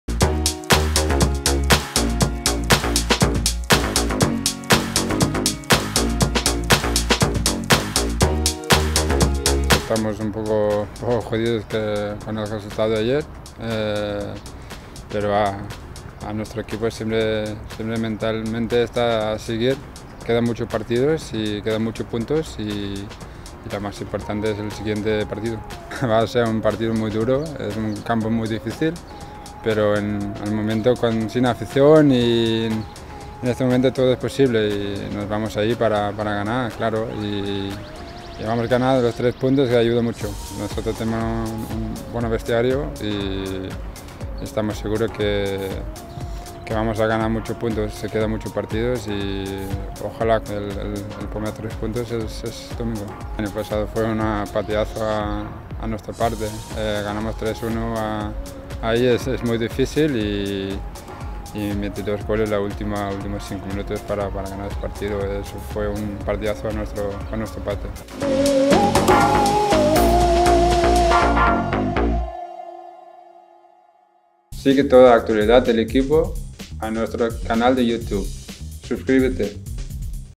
🎤📹DANIEL WASS HABLA EN EXCLUSIVA PARA VCF MEDIA
El internacional danés está convencido que el equipo sumará “muchos puntos en lo que falta de temporada”.

The Danish international is confident the team will earn"many points in the remainder of the season".